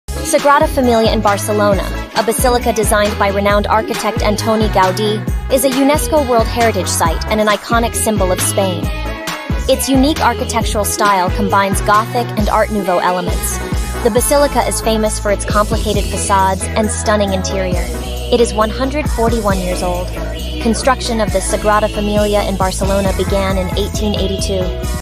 🇪🇸 Spain: Sagrada Familia Barcelona
Construction of the Sagrada Família in Barcelona began in 1882, so as of 2023, it is 141 years old. However, the basilica is still under construction, and its completion is estimated to occur in the next few decades. The ongoing construction work is a testament to the complexity and ambition of Antoni Gaudí's vision for this remarkable architectural masterpiece.